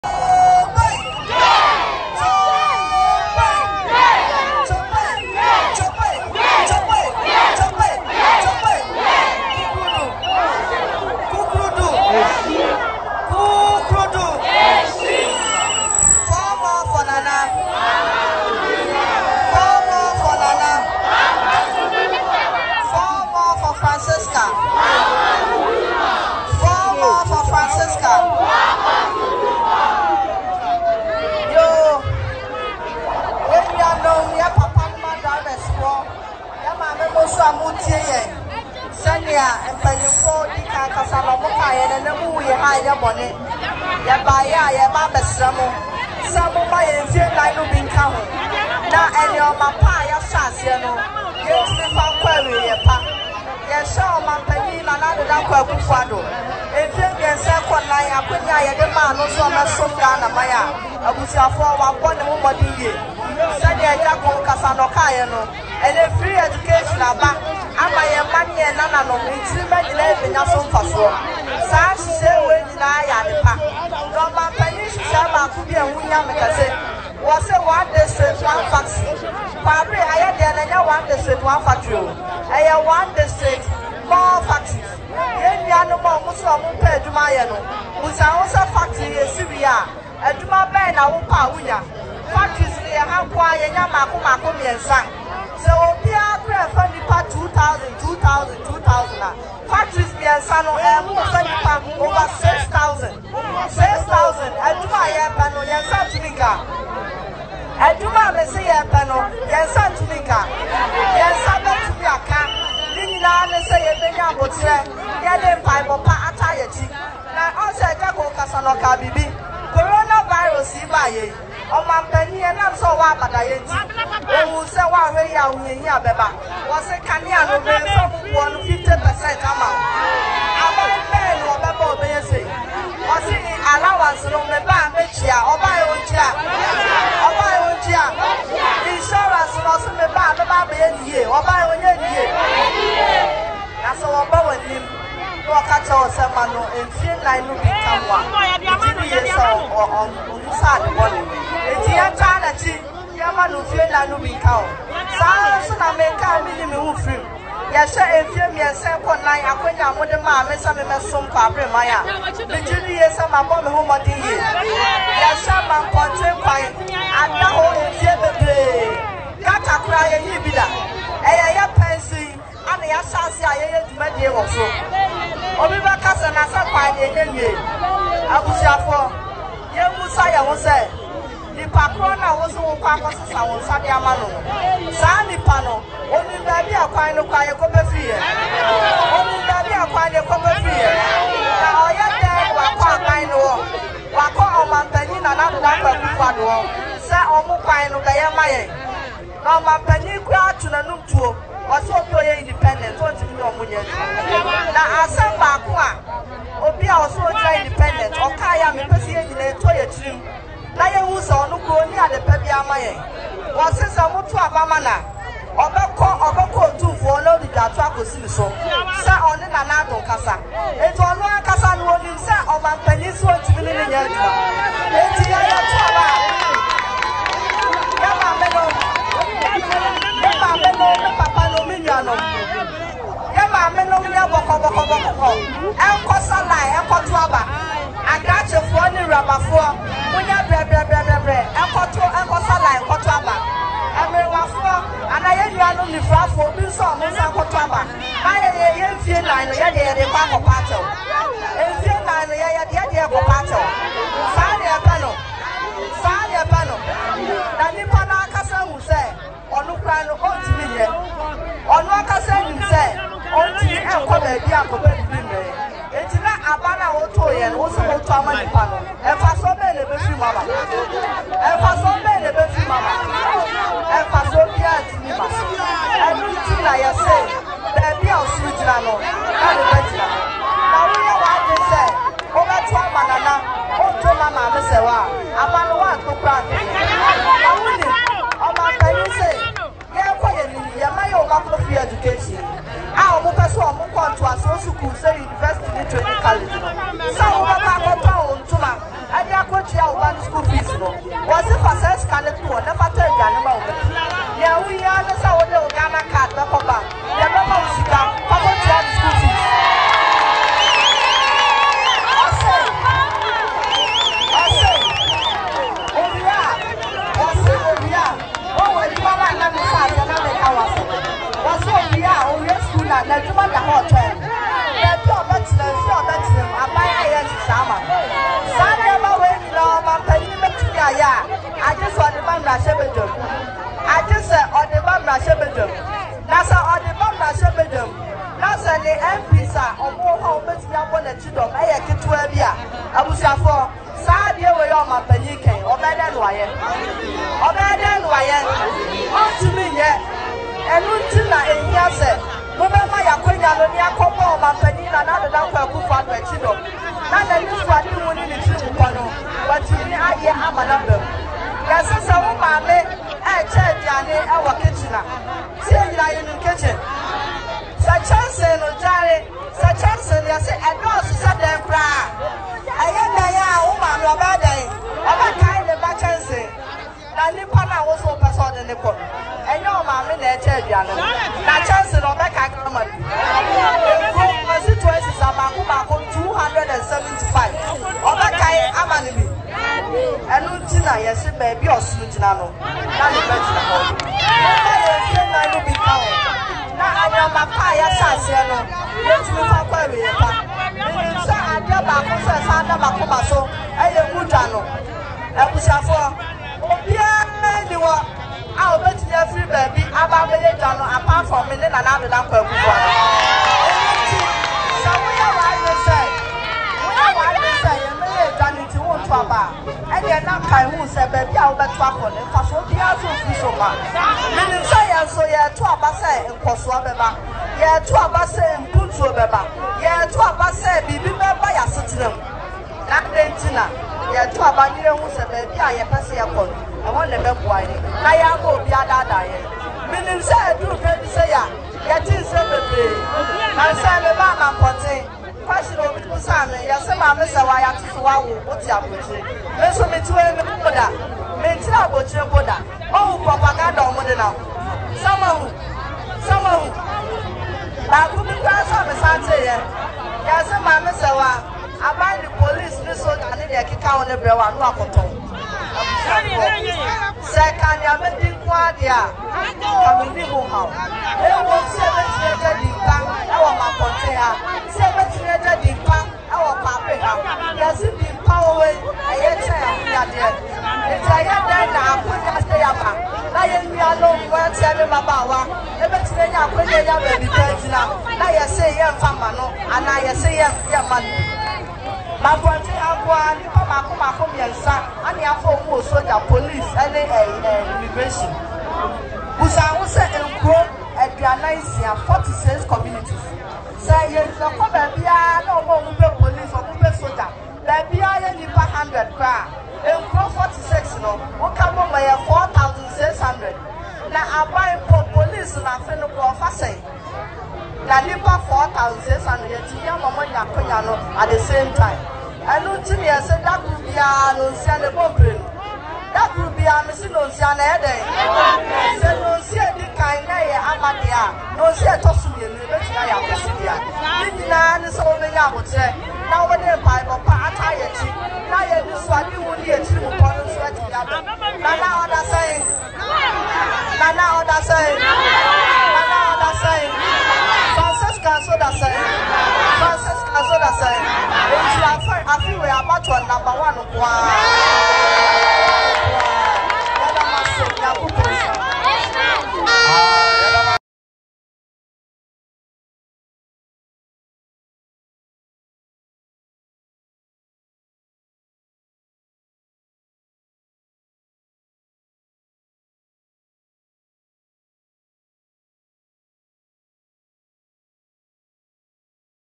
Dr Kwaku Oteng’s daughter; MP for Kwabere Mamponten(Francisca Oteng Mensah) campaigns massively
Francisca Oteng Mensah, aged 27 is the youngest member of Parliament in Ghana. She is the daughter of Dr Kwaku Oteng the famous business man in Ghana. She is the current member of Parliament for Kwabere and also the Parliamentary candidate. She went unopposed during the NPP parliamentary elections.